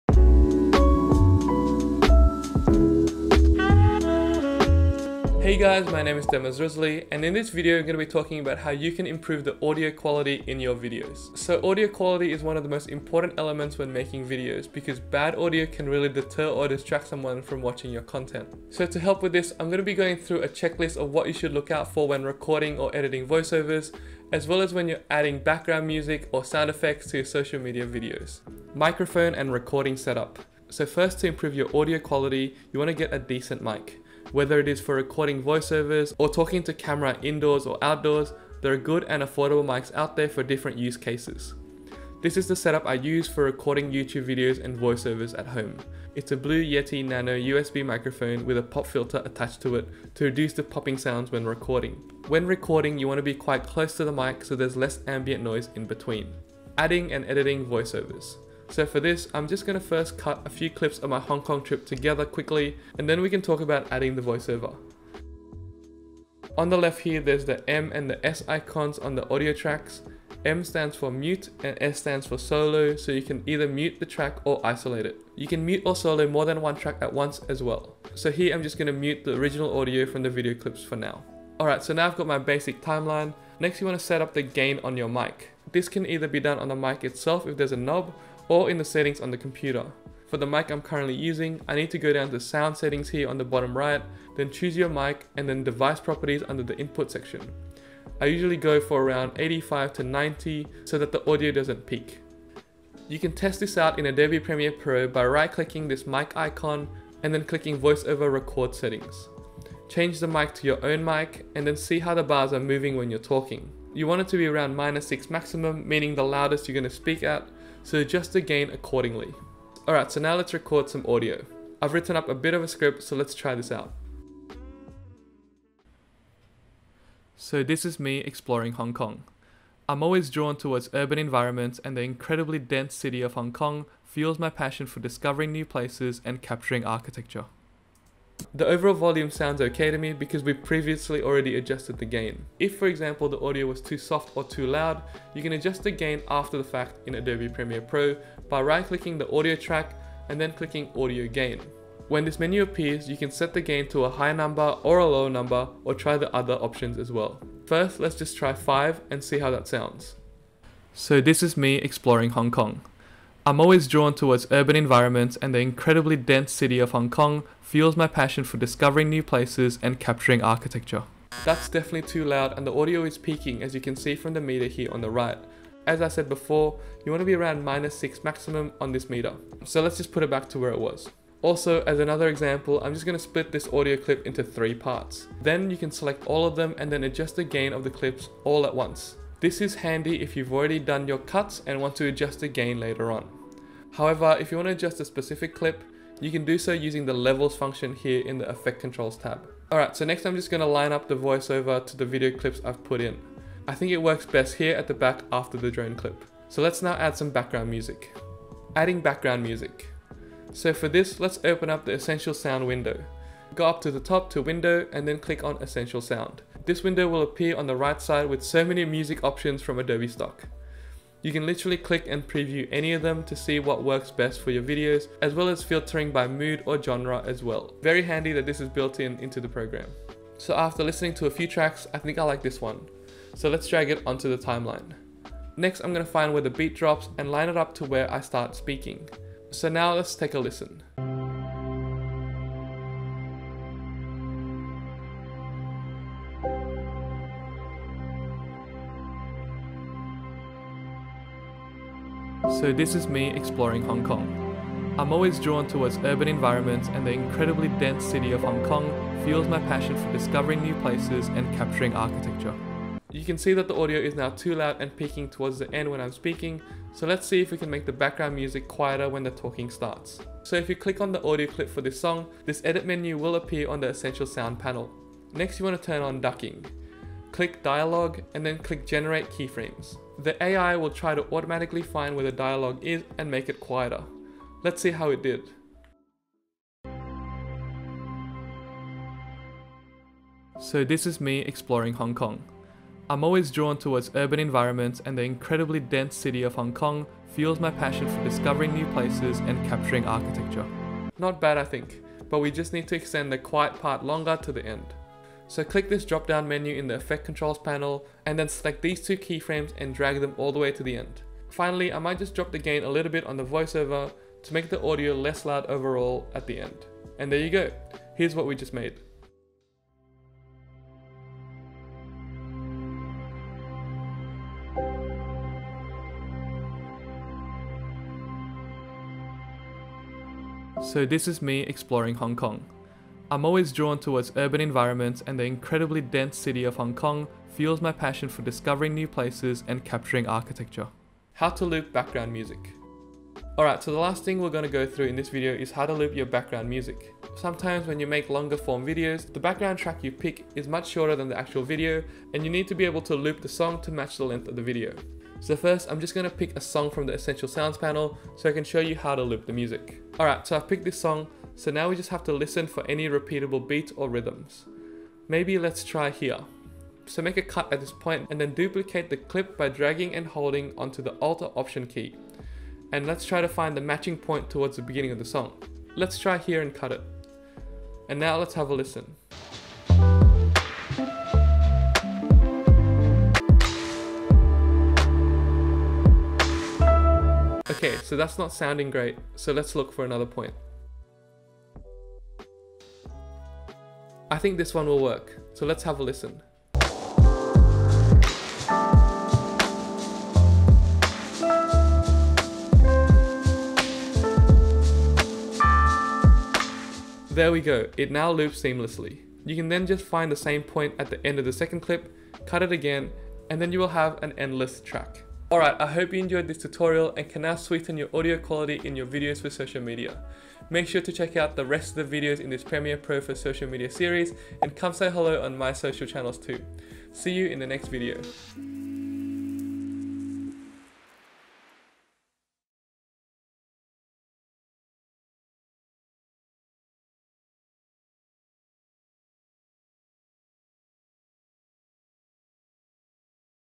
How to improve audio quality on your videos | Adobe Premiere Pro
In this tutorial we're going to show you how to improve audio quality in Adobe Premiere Pro​
In this Adobe Premiere Pro Tutorial, you will learn about ways you can improve the audio quality of your video. From setting up your microphone for voiceovers to adjusting the background music’s volume, follow along step by step as @Demas shares tips for making your video audio sound crisp and clear.

Here are the tips and features covered in this video​:
​0:00 Improve your video audio quality
0:28 Have a good microphone and recording setup
1:08 Mute or isolate tracks
1:23 Set up and adjust audio gain
3:30 Add Background Music from Adobe Stock
4:23 Turn on Ducking and click “dialogue” to soften the background music
5:55 Loop an audio clip

►Add titles and motion graphics to social media videos | Adobe Premiere Pro